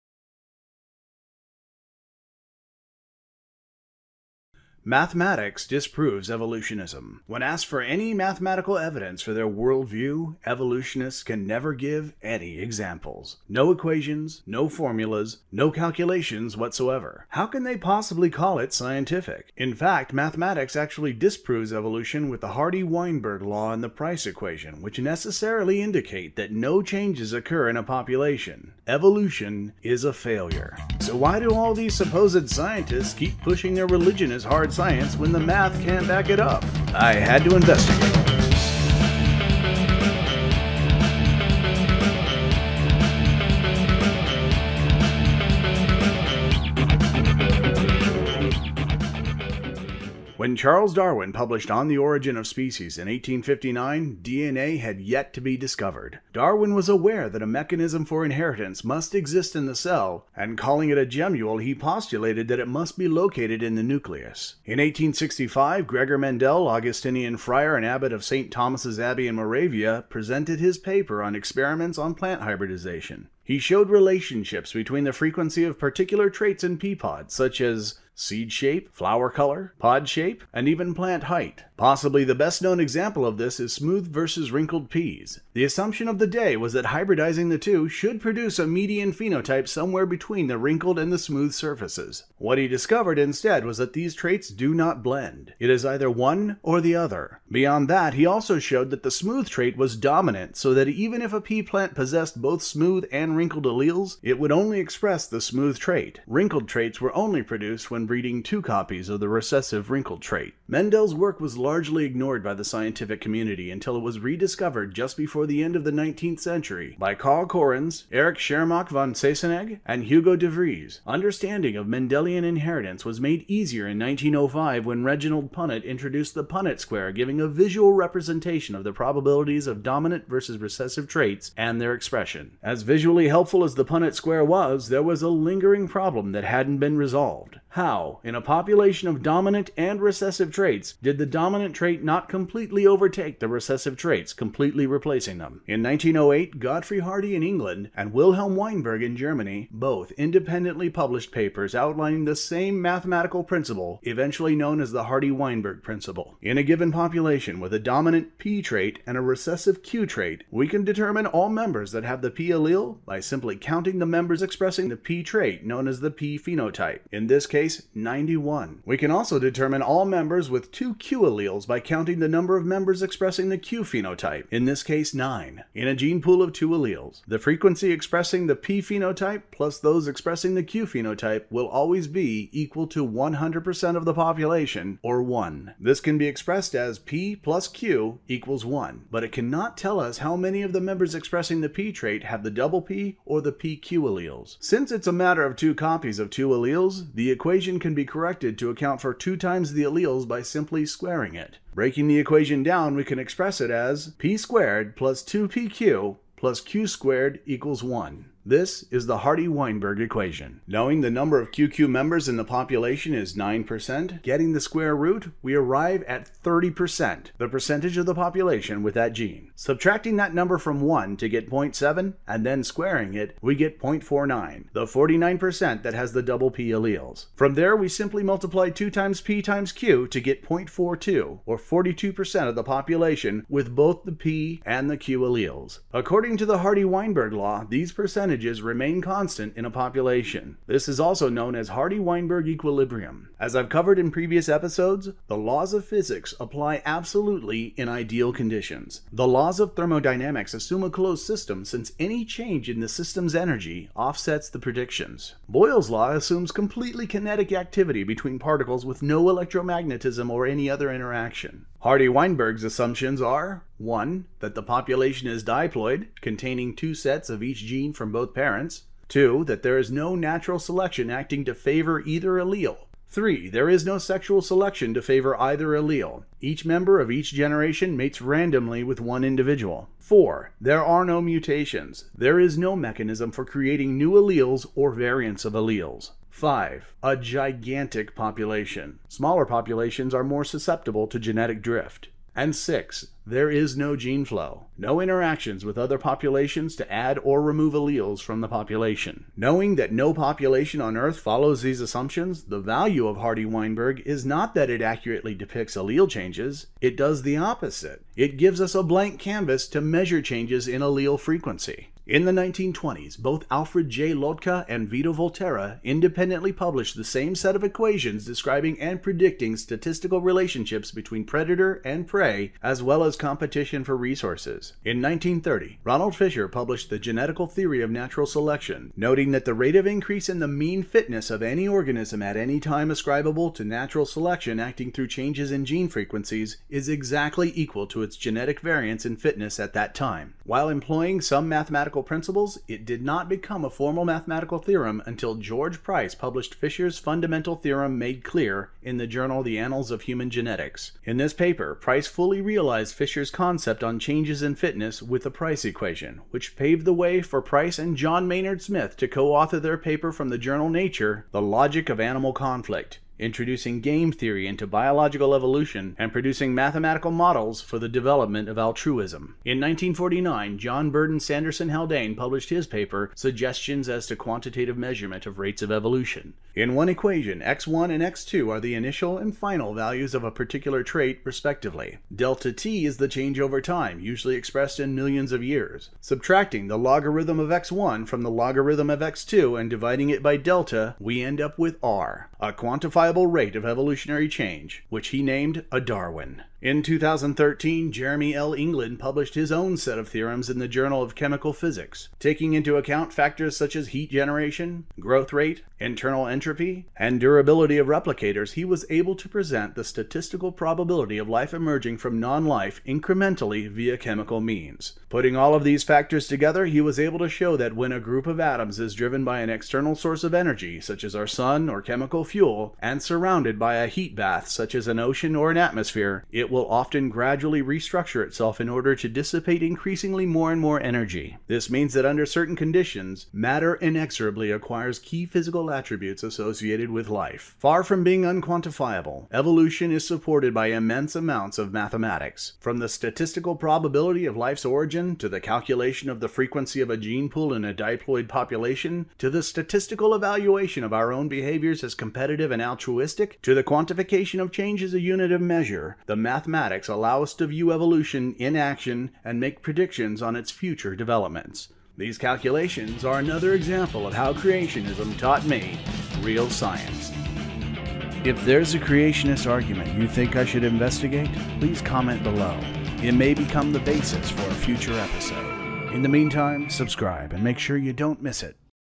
How Creationism Taught Me Real Science 78 Evolutionist Maths
In this episode we examine the possibility (or impossibility) of evolution by studying what the maths have to say.

Darwin Discussing the Gemmule Darwin, C. R. October 1869. Pangenesis.—Mr. Darwin's reply to Professor Delpino. Scientific Opinion: A Weekly Record of Scientific Progress at Home & Abroad 2

Alfred J. Lotka's derivations of the Lotka-Volterra equations.

Vito Volterra's derivations of the Lotka-Volterra equations.

George Price and John Maynard "The logic of animal conflict"

John Burdon Sanderson Haldane "Suggestions as to quantitative measurement of rates of evolution"

Jeremy England - Abiogenesis Calculations
Journal Paper

Update on Jeremy England's work.